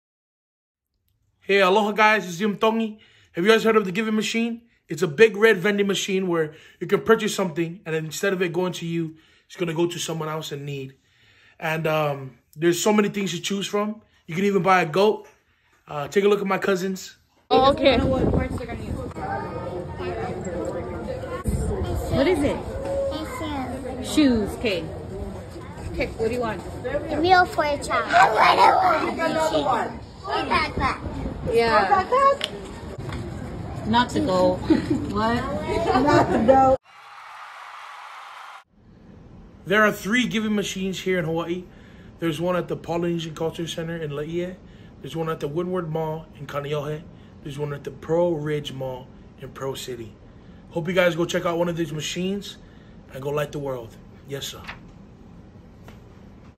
LightTheWorld! Give a gift from a Giving Machine
A Giving Machine is where you get a gift for someone in need. Go check one out near you and Light The World! Yessah 🤙🏾
Here are the locations in Hawaii 1) Laie- The Polynesian Cultural Center 2) Kaneohe- Windward Mall or 3) Pearl City- Pearlridge Mall